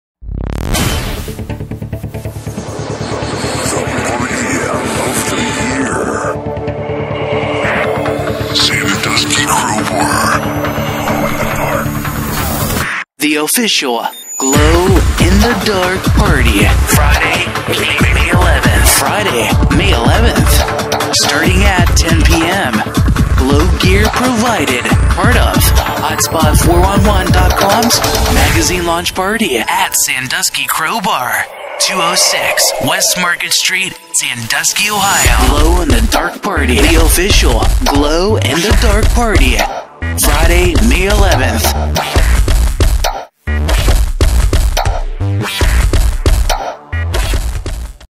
May 11, 2012 The official glow n-the dark party @ Sandusky
The Official Glow N-The Dark Party
Glow Gear Provided
Party starts at 10 pm
Wear your glow gear (white) black light party
Sandusky Crowbar
206 West Market Street - Sandusky, Ohio 44870